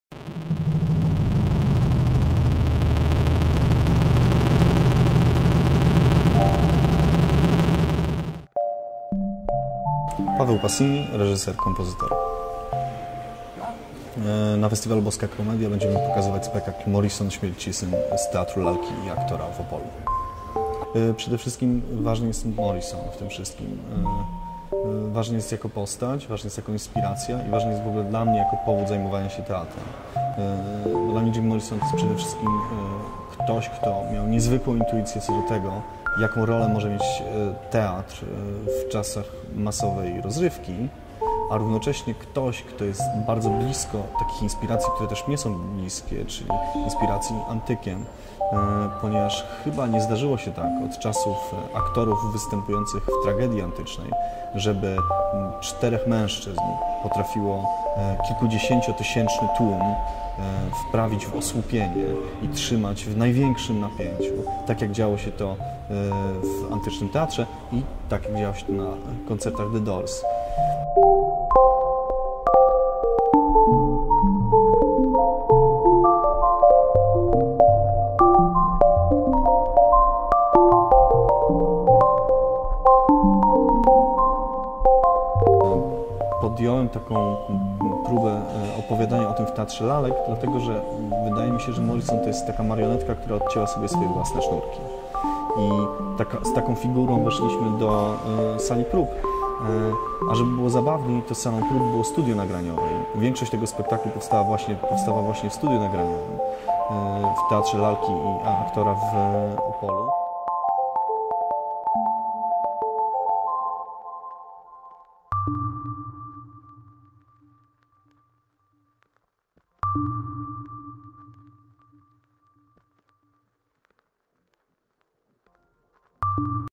FESTIWAL BOSKA KOMEDIA - Paweł PASSINI
Paweł Passini zaprasza na spektakl "MORRISON/ŚMIERCISYN" Opolskiego Teatru Lalki i Aktora im. Alojzego Smolki, prezentowanego w ramach 6. MIĘDZYNARODOWEGO FESTIWALU TEATRLANEGO BOSKA KOMEDIA w Krakowie.